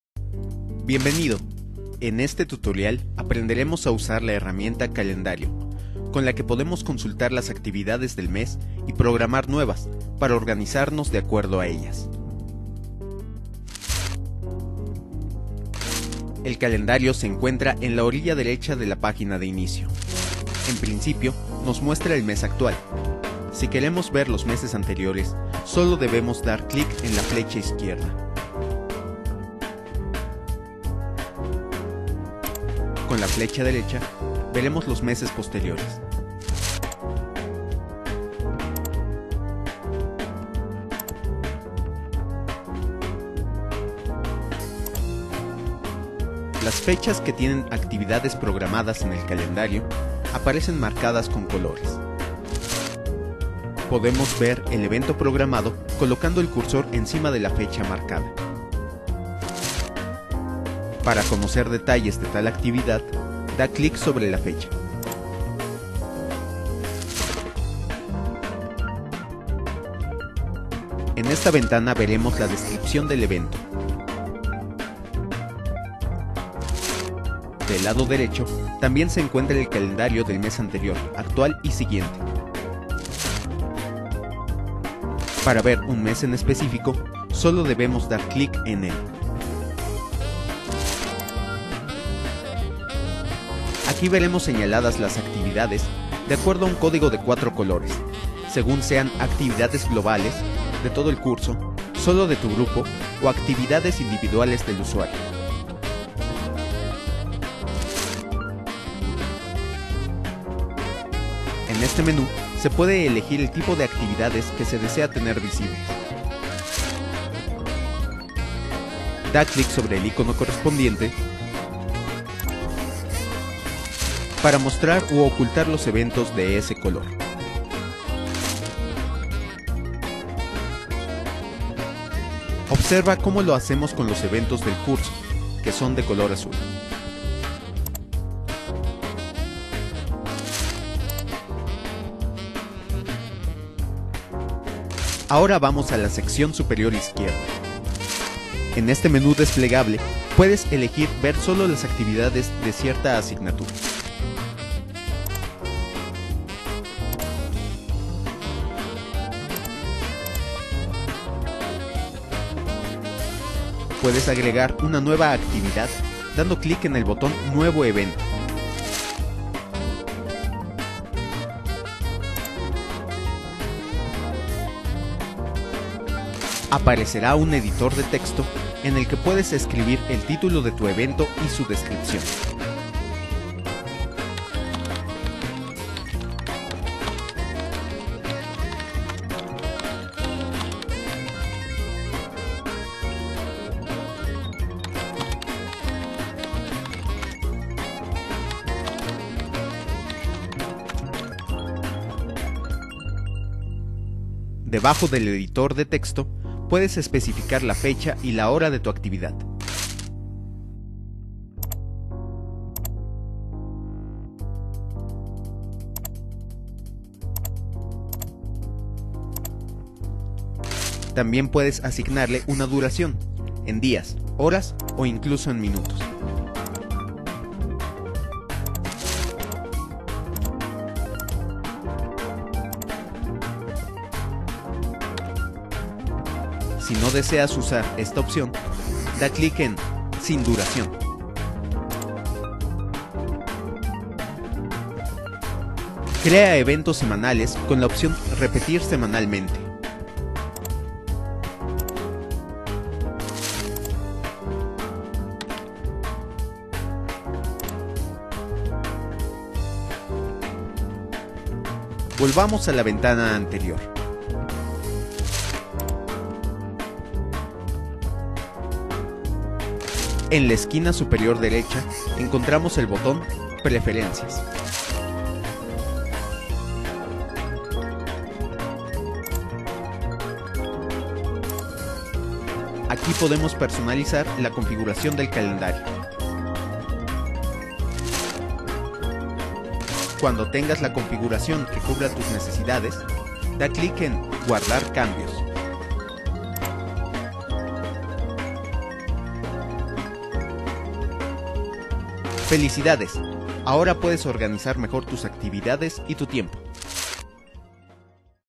Calendario
En este tutorial aprenderemos a usar la herramienta Calendario, con la que podemos consultar las actividades del mes y programar nuevas, para organizarnos de acuerdo a ellas.